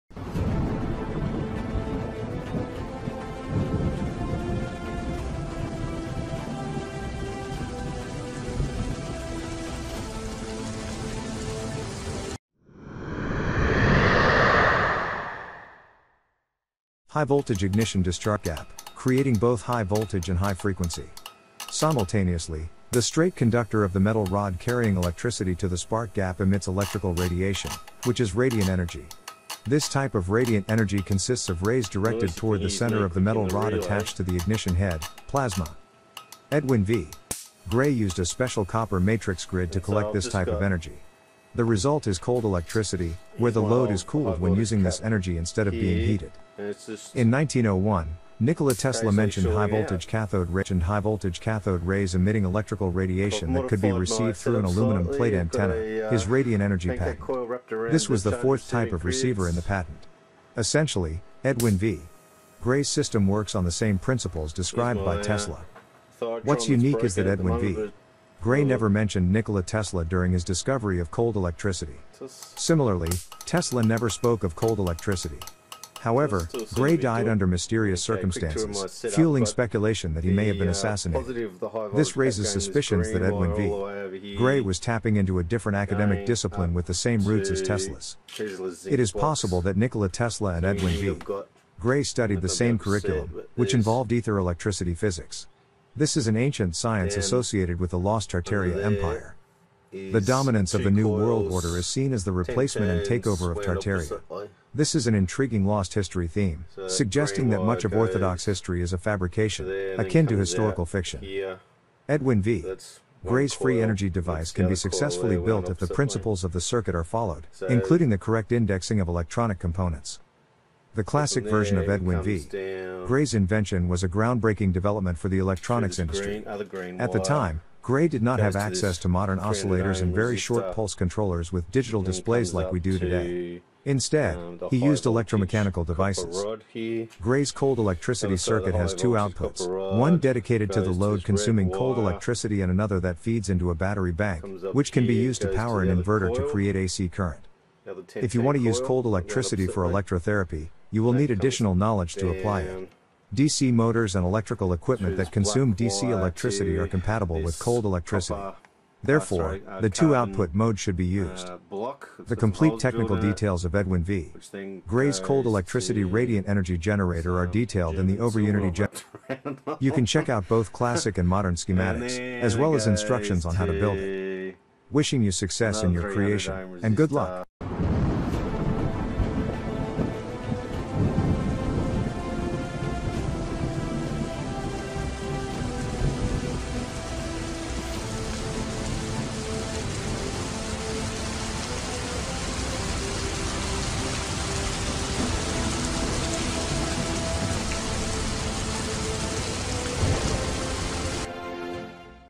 Edwin V. Gray's Cold Electricity vs. Tesla’s Radiant Energy: A Forgotten Legacy
Building Gray's Cold Electricity Device - A Dive into Free Energy Tech

Edwin V. Gray's groundbreaking invention of "cold electricity" using radiant energy. By harnessing high-voltage plasma discharges through a spark gap, Gray collected this unique energy type with a copper matrix grid, leading to a cooling effect on electrical loads rather than heating them. His work parallels Nikola Tesla's earlier discoveries of radiant energy but remains distinct, adding mystery to Gray's untimely death. The video explores how Gray’s cold electricity technology can be applied today, from powering DC motors to electrotherapy, and offers insights into building his free energy device